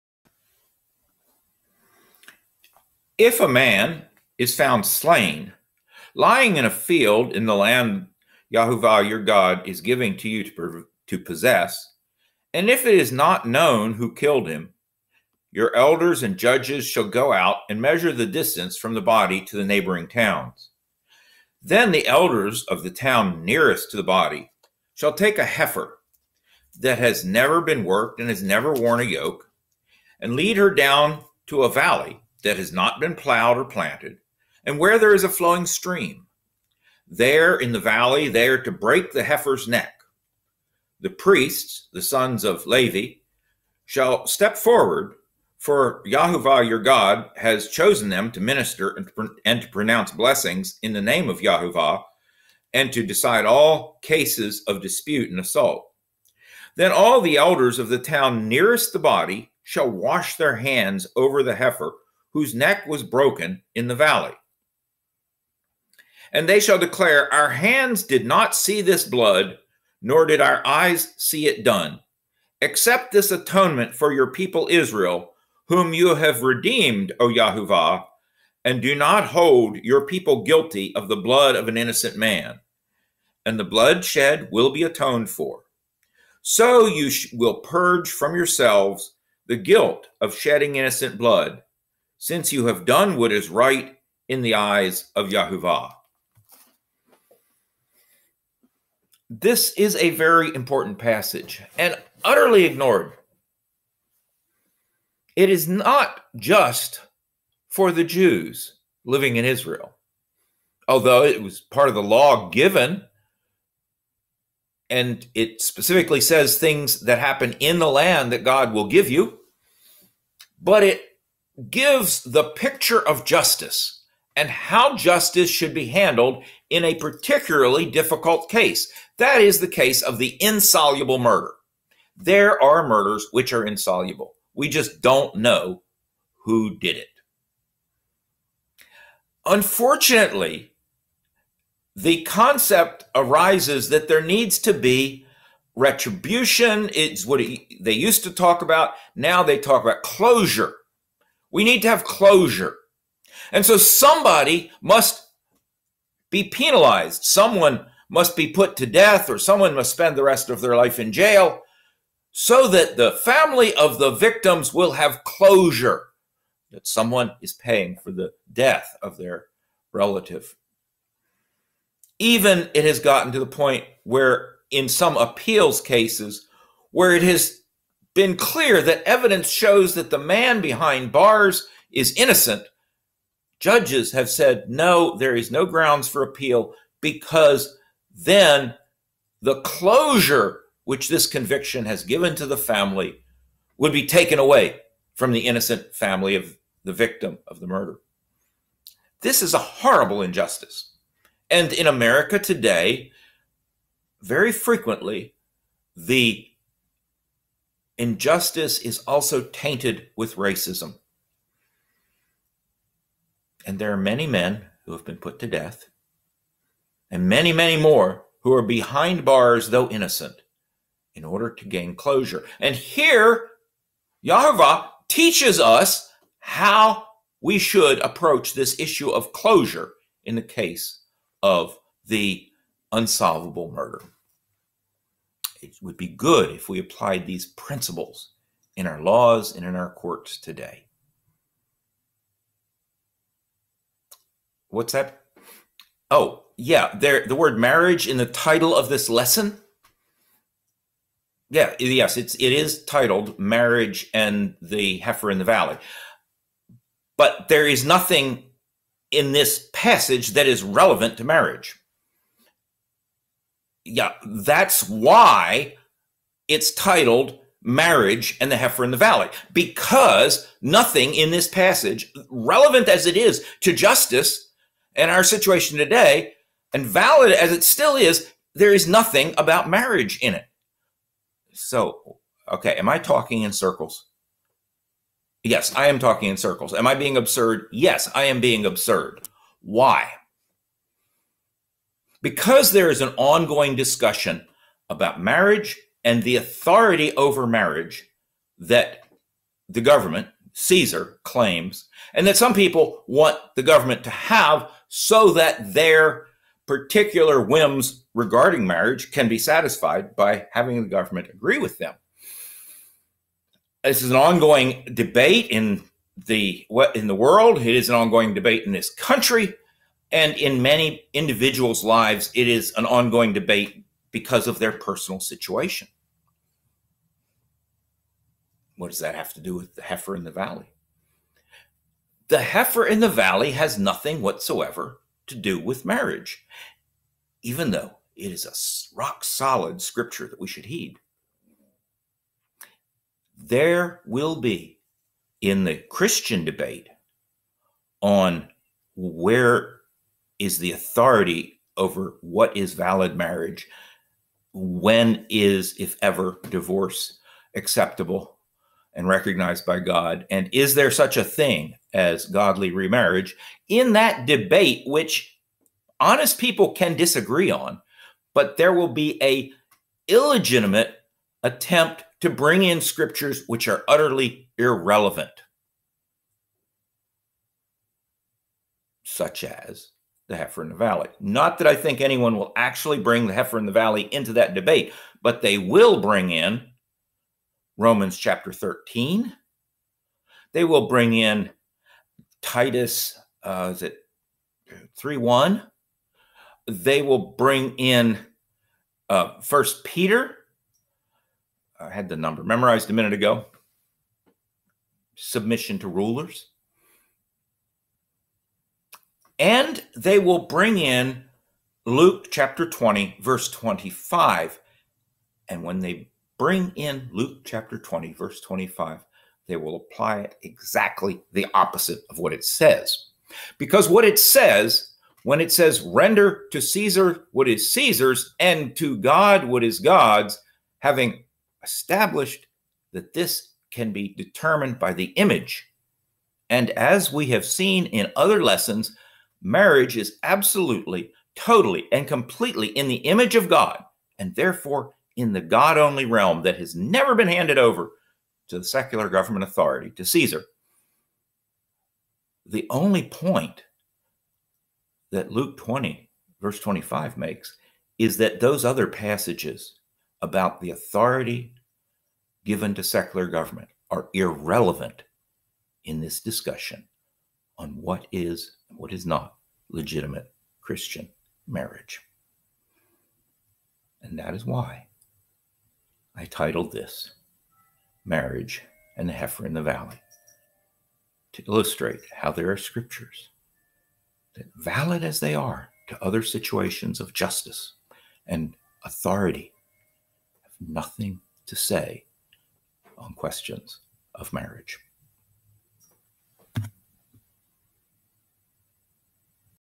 Deuteronomy 21:1-9 - Marriage, and the Heifer in the Valley
“If a slain person is found lying in the open country in the land which the Lord your God gives you to possess, and it is not known who has struck him,  then your elders and your judges shall go out and measure the distance to the cities which are around the slain one.  It shall be that the city which is nearest to the slain man, that is, the elders of that city, shall take a heifer of the herd, which has not been worked and which has not pulled in a yoke;  and the elders of that city shall bring the heifer down to a valley with running water, which has not been plowed or sown, and shall break the heifer’s neck there in the valley.  Then the priests, the sons of Levi, shall come near, for the Lord your God has chosen them to serve Him and to bless in the name of the Lord; and every dispute and every assault shall be settled by them.  All the elders of that city which is nearest to the slain man shall wash their hands over the heifer whose neck was broken in the valley;  and they shall answer and say, ‘Our hands did not shed this blood, nor did our eyes see it.  Forgive Your people Israel whom You have redeemed, O Lord, and do not place the guilt of innocent blood in the midst of Your people Israel.’ And the bloodguiltiness shall be forgiven them.  So you shall remove the guilt of innocent blood from your midst, when you do what is right in the eyes of the Lord.

Deuteronomy 21:1-9 has absolutely no bearing whatsoever on the Christian doctrines of marriage.
Neither do 1 Peter 2:13-17, Titus 3:1, nor Romans 13:1-7 have any bearing on this subject.

An Inconvenient Truth